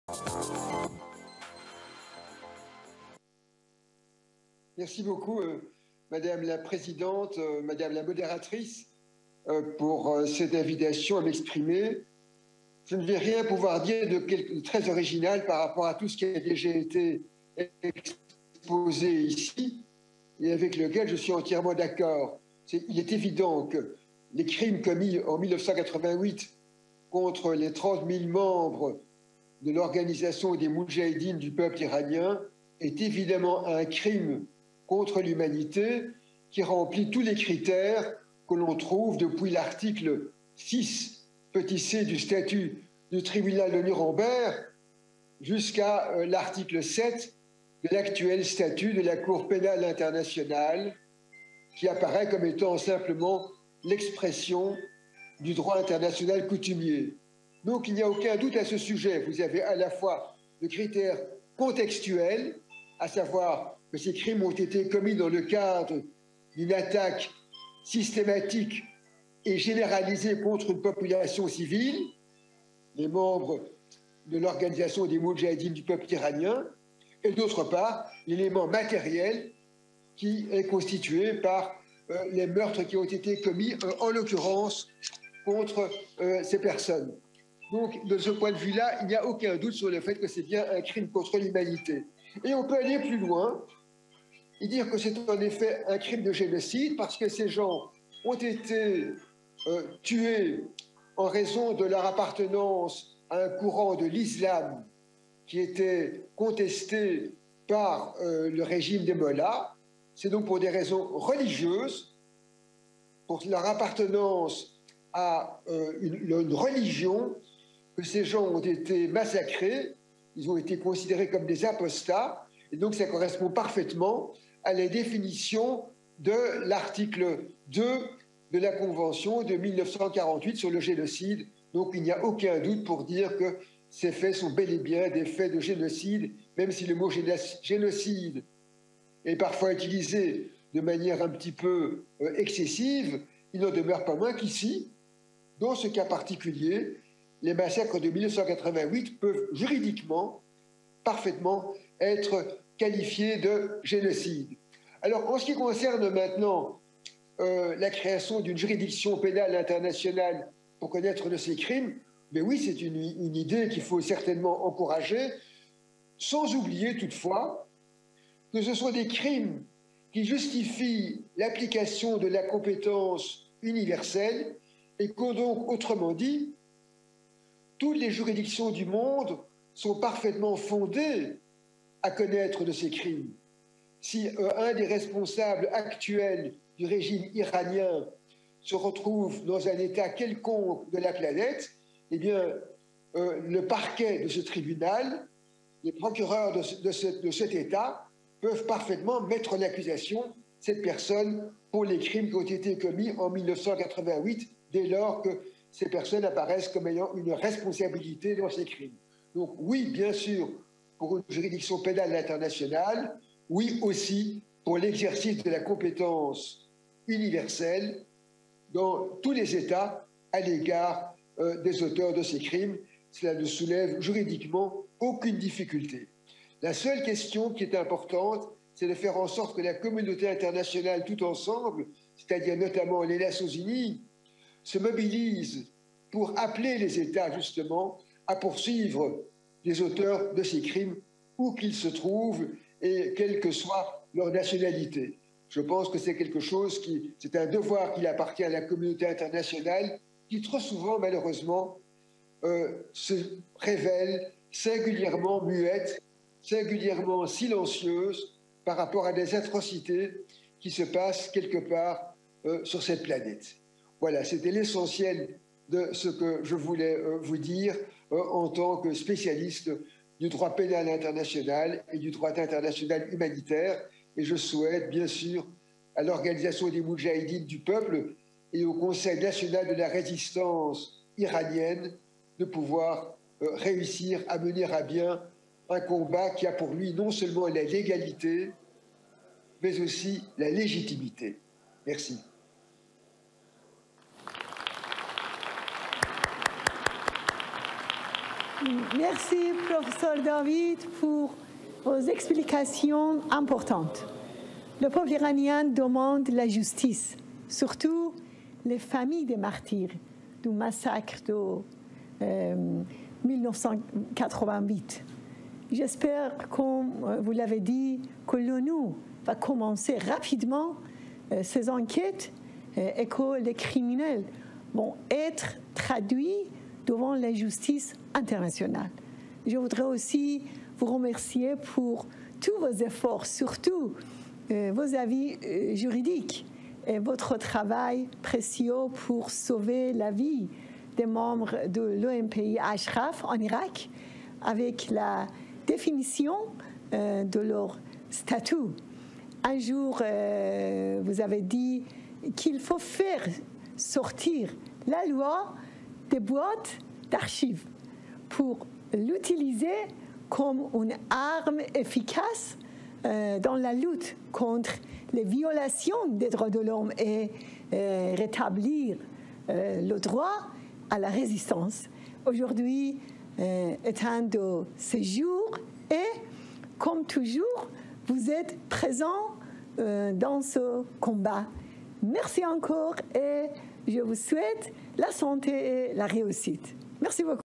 210827_Eric David, Professor of International Law from Belgium on the subject of 1988 Massacre
There is no doubt that this is a crime against humanity. It was an attack on a civil population. This is also a genocide because the victims were killed based on their adherence to a set of beliefs that was banned by the mullahs.

All international jurisdictions can try this crime. If a regime official is in any country, that country can easily accuse and try that person for his responsibility in the 1988 massacre. We have no difficulty judicially bringing this case to court.

We have to reiterate that these criminals be brought to justice in international courts.